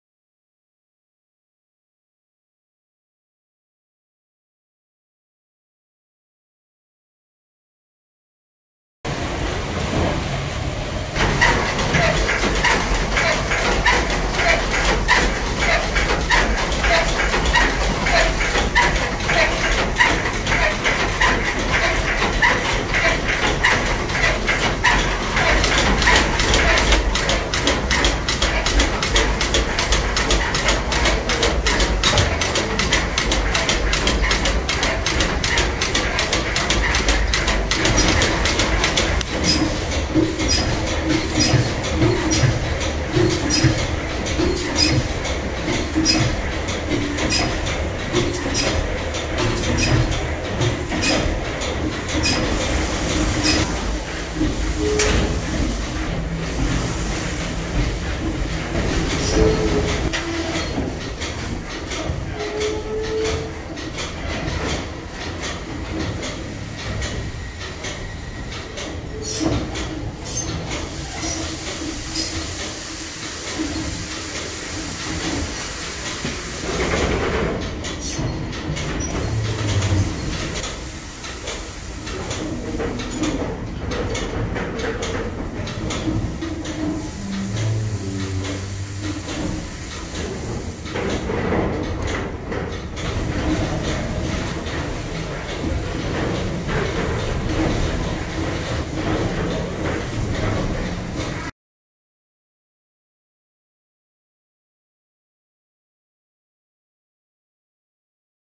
In Steam Movie
Maid Of The Loch Paddle Steamer .Engine Room Video